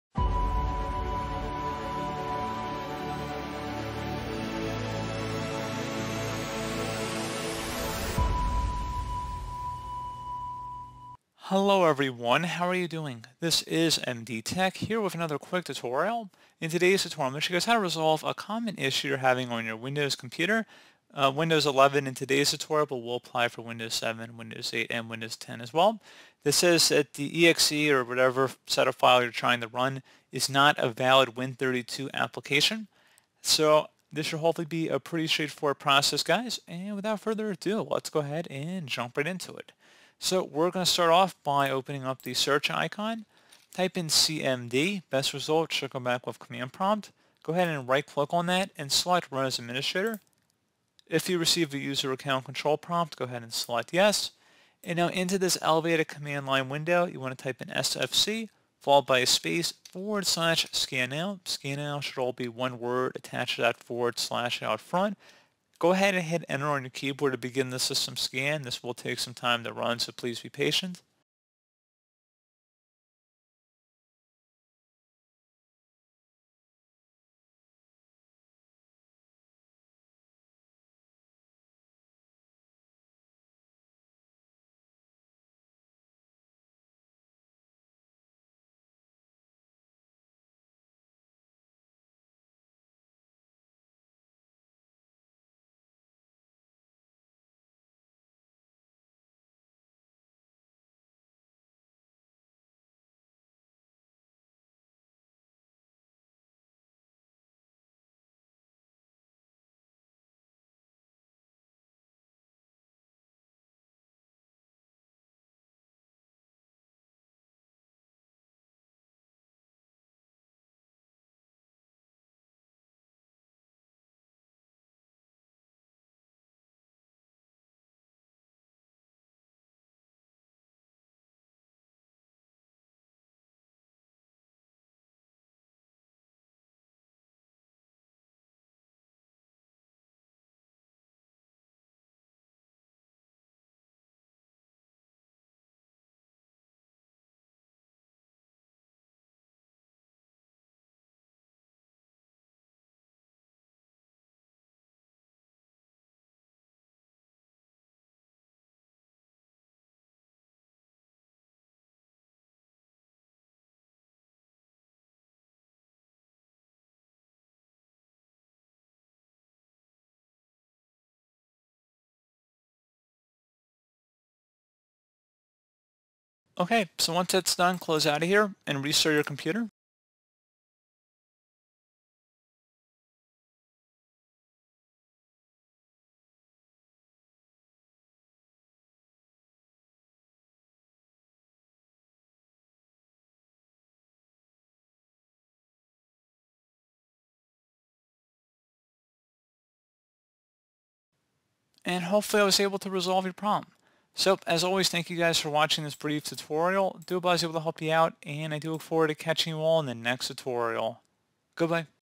How to Fix Exe Not a Valid win32 Application In Windows 11 [Solution]
Occasionally you may get an error that a problem on Windows is "not a valid Win32 application." This message may seem confusing, but it simply means that your version of Windows isn't able to run the app since it can't understand what's in the file. To fix it, reacquire the file from the place you got it.

Issues addressed in this tutorial:
exe not a valid win32 application
exe is not a valid win32 application
exe is not a valid win32 application windows 11

If the file or program you're attempting to run is corrupt or missing, Windows cannot execute the file properly and sometimes generate the error message above.